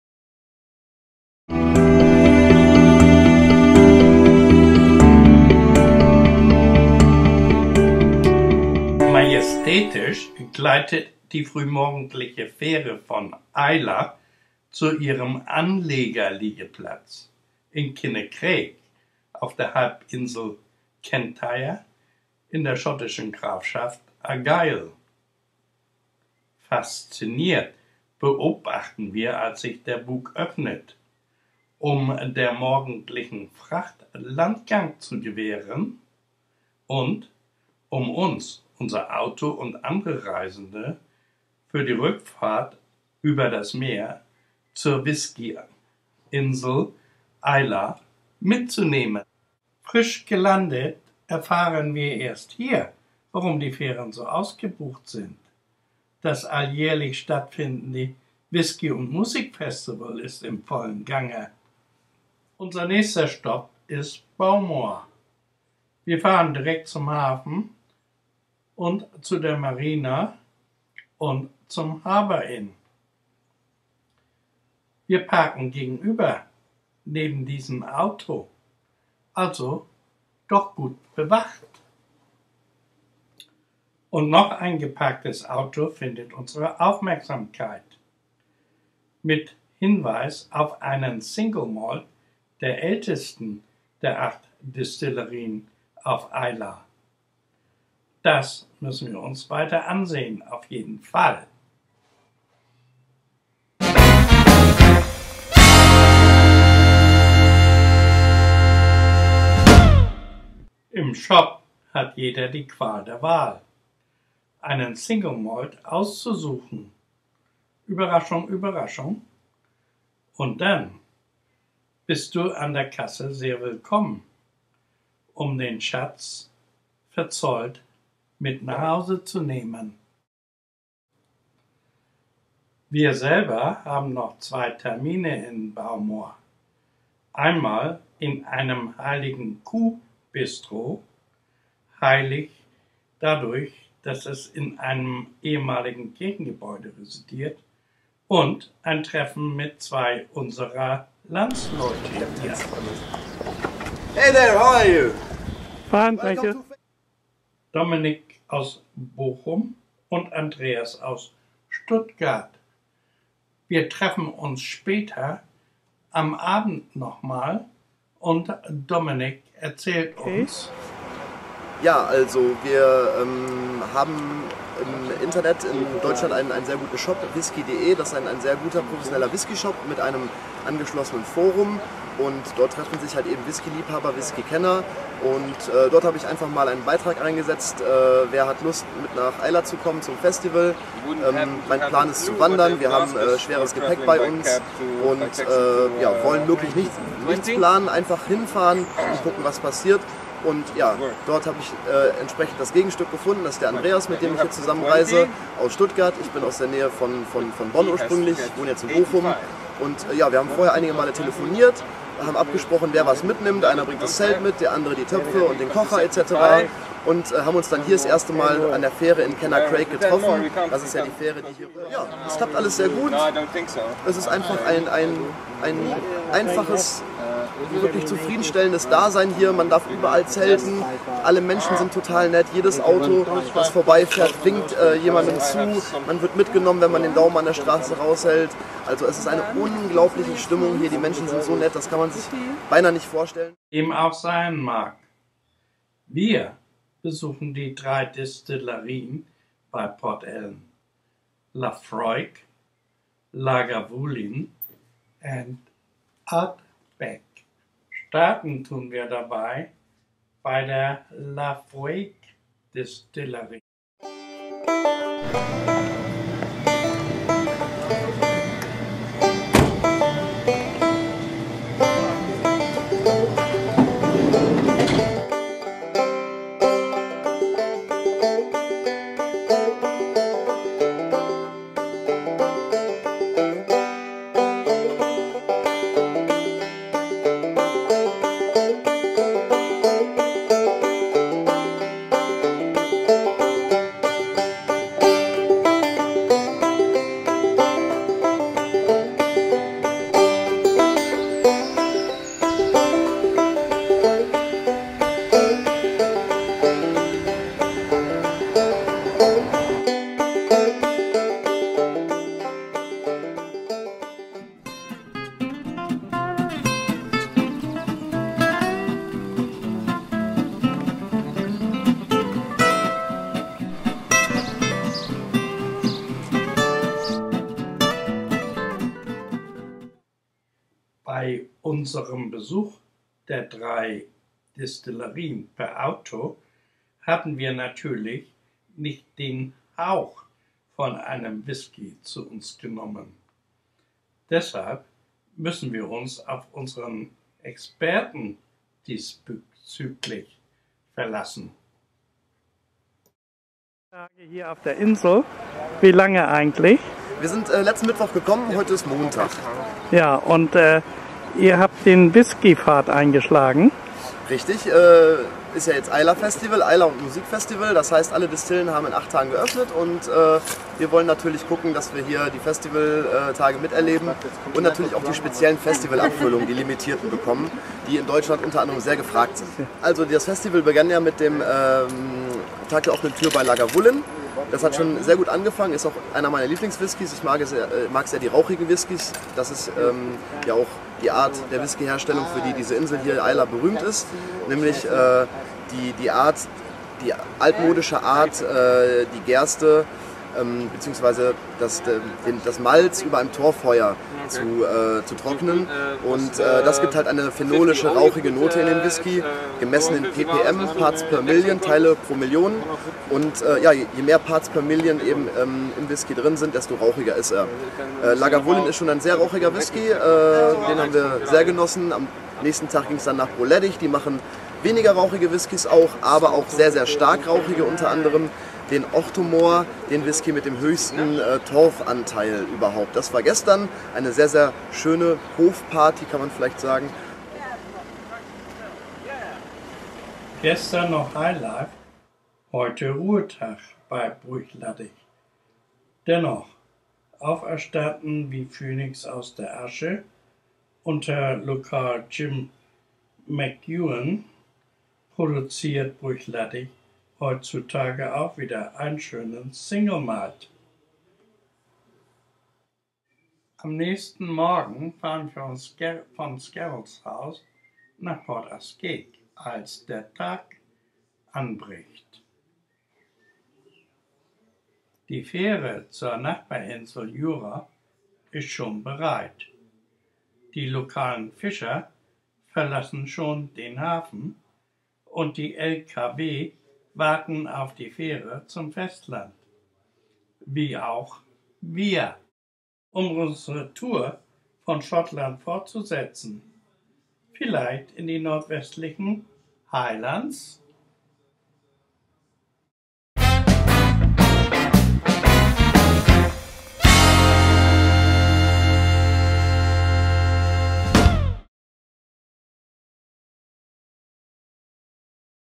Auf nach Schottland - 3. Islay
Eine Überfahrt und ein eintägiger Aufenthalt auf der Whisky Insel Islay zur Festival Zeit.
Unser Whisky Experte klärt Sie auf über Single Malts und deren rauchige Torfanteile in ppm.